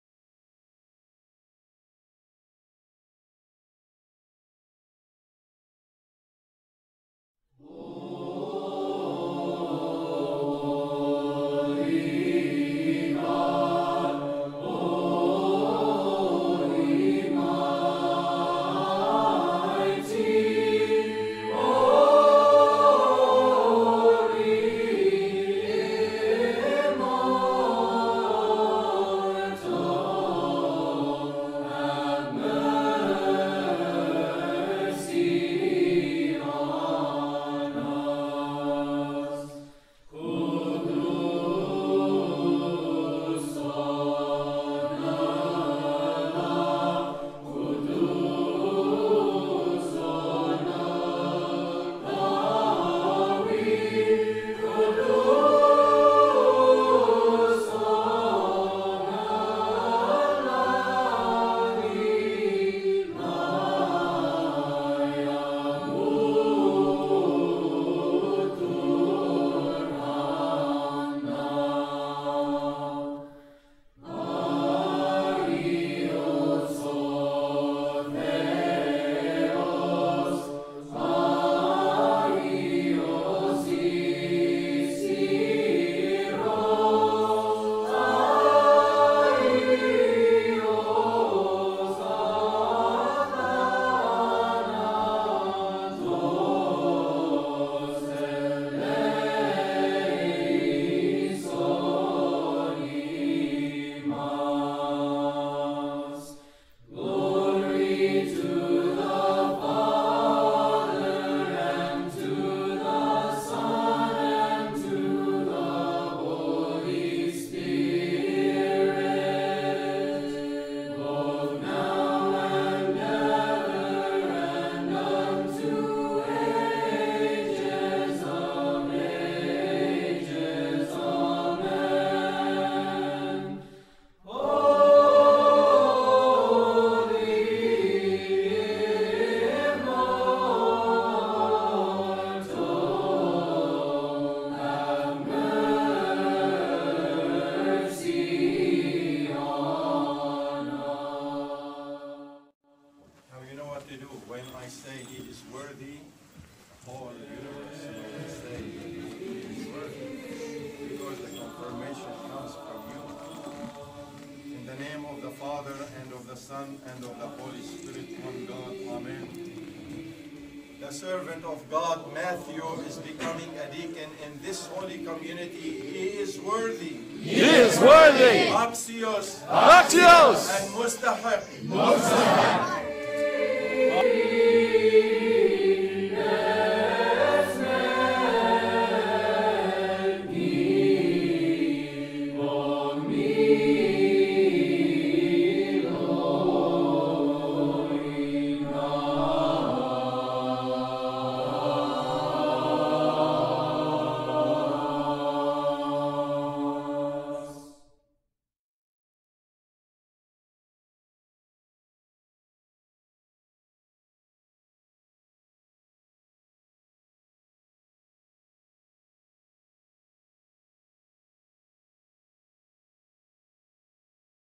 Metropolitan JOSEPH visit to Holy Transfiguration on December 13th, 2018
Elevation of Father Mark to Archpriest
Ordination of Subdeacon Matthew to the Holy Diaconate
Blessing of Stephen to the Subdiaconate
Tonsuring of Sergius as a Reader